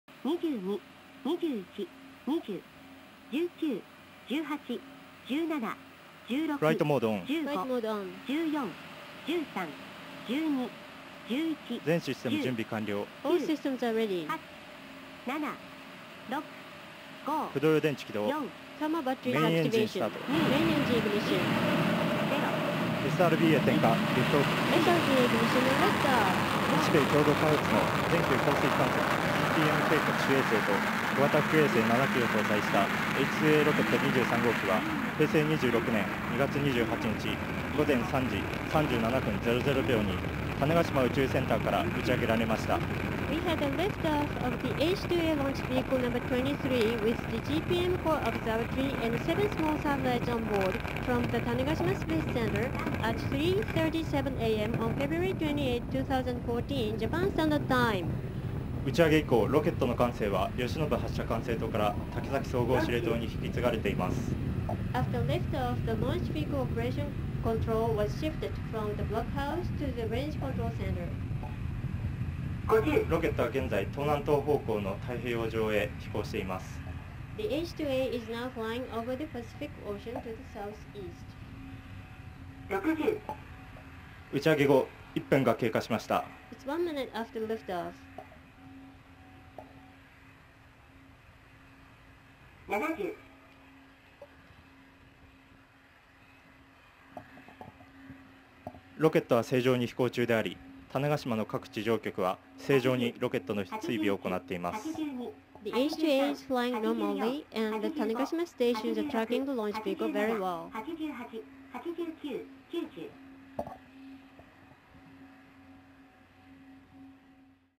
NASA | GPM Rocket Launch
A Japanese H-IIA rocket with the NASA-Japan Aerospace Exploration Agency (JAXA), Global Precipitation Measurement (GPM) Core Observatory onboard, is seen launching from th Tanegashima Space Center, Friday, Feb. 28, 2014, Tanegashima Space Center. The GPM spacecraft will collect information that unifies data from an international network of existing and future satellites to map global rainfall and snowfall every three hours.